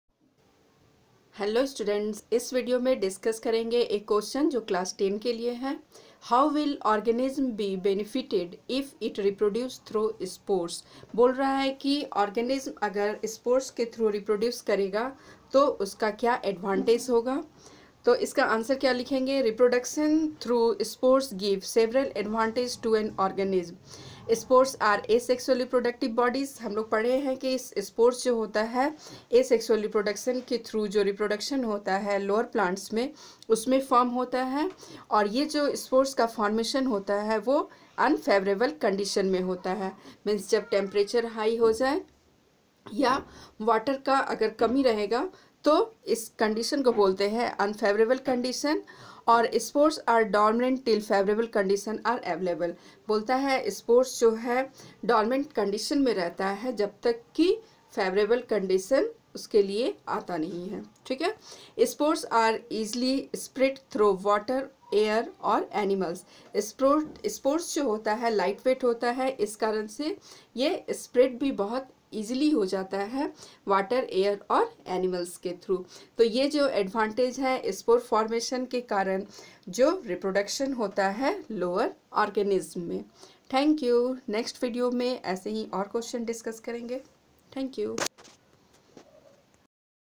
How will organism be benefited if it reproduce through spores ?
Biology for class 10 discuss NCERT question How will organism be benefited if it reproduces through spores?